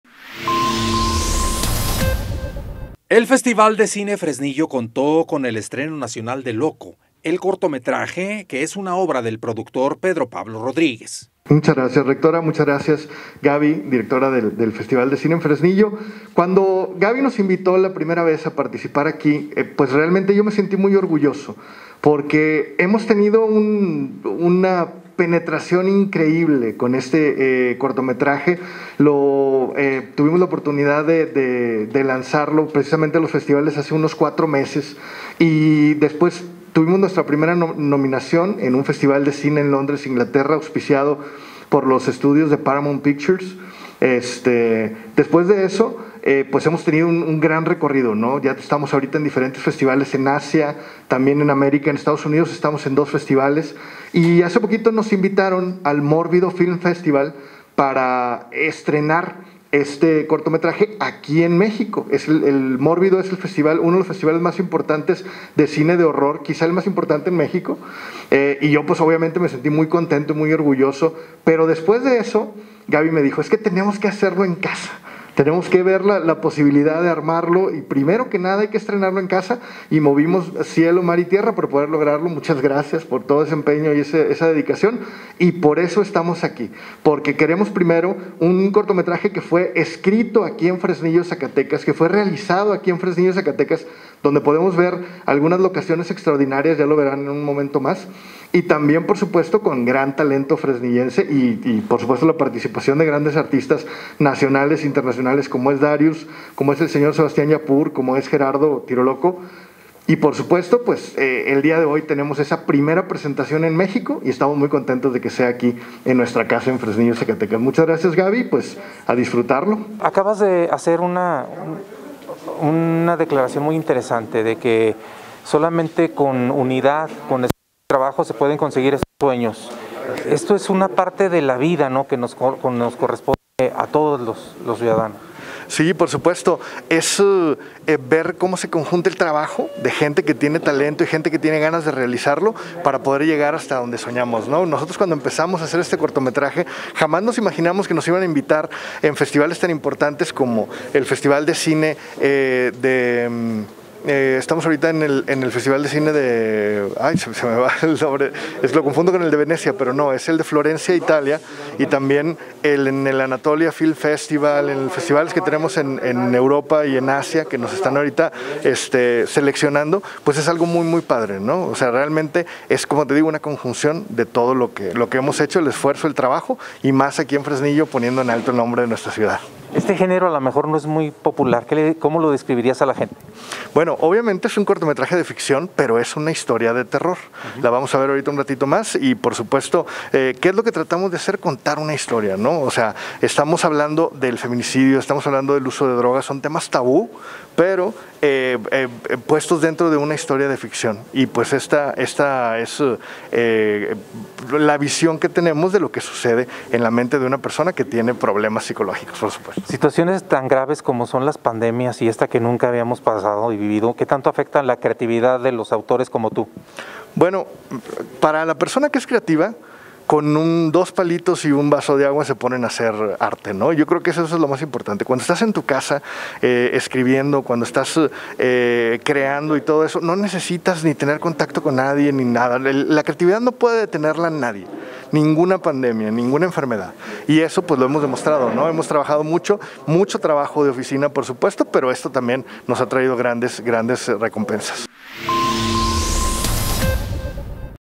Festival de Cine en Fresnillo estrena el cortometraje ‘Loko’
'Loko', cortometraje producido por Dharius, protagonizado por Gerardo Elizondo 'Tiro Loko' y en dirección de Pedro Pablo Rodríguez, el cual habla del gran esfuerzo y dedicación  para realizar este proyecto  y que cuenta con la participación del actor de doblaje Sebastián Llapur y que ha tenido nominaciones en grandes festivales en Europa.